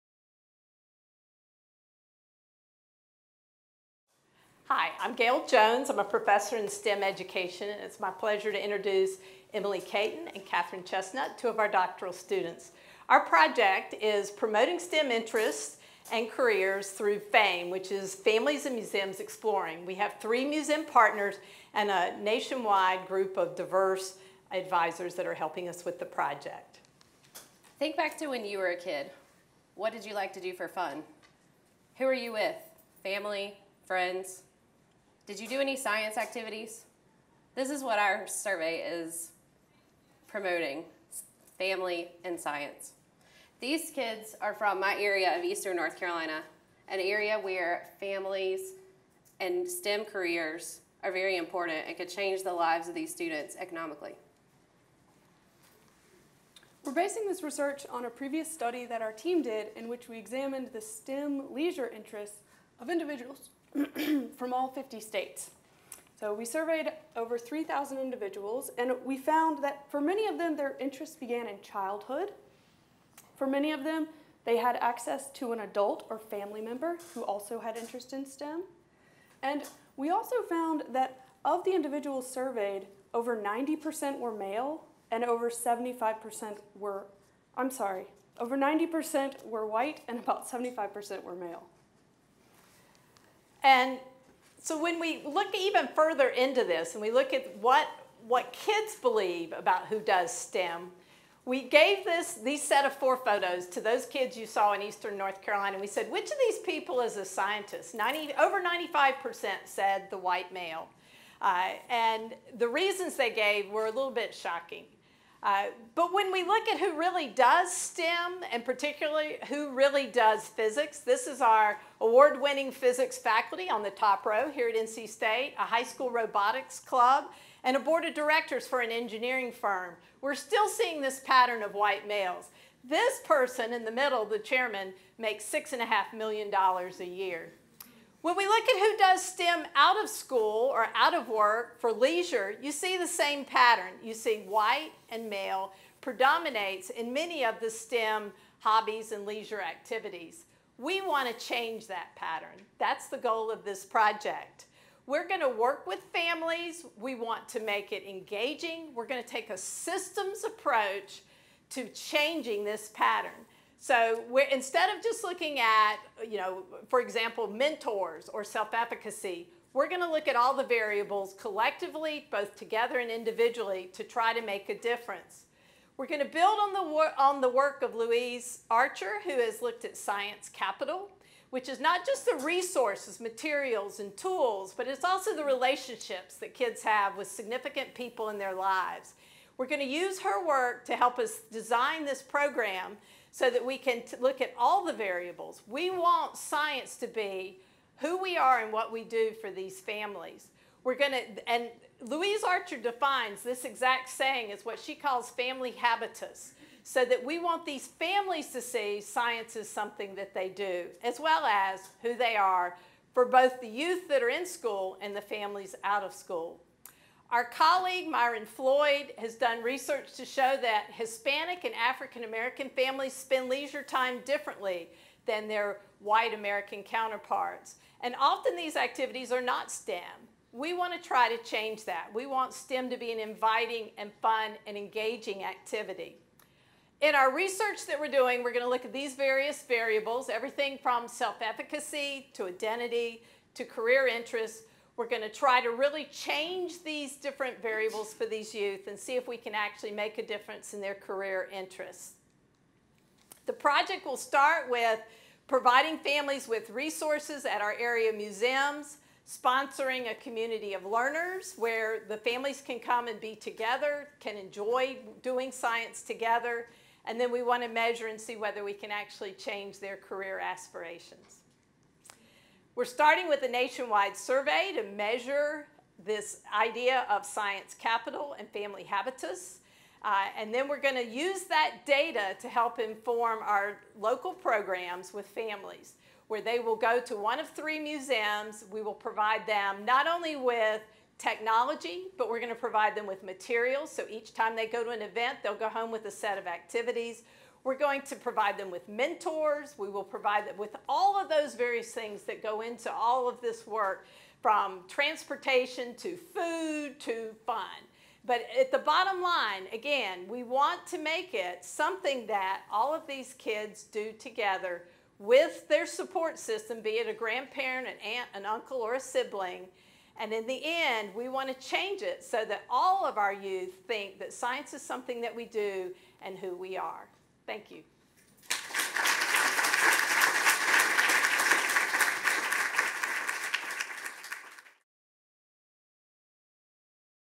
Gail Jones, Ph.D. | Promoting STEM Interests and Careers through FAME
Science is something anyone can do. So why do so many children and youth believe scientists, based on surveys, are largely white males? And how can we change the perceptions and patterns? Gail Jones, a professor of science education at the NC State College of Education, discusses ways she's working to do that during a presentation at the College of Education's Spring 2017 All College Meeting with Emily Cayton and Katherine Chesnutt, graduate students in science education.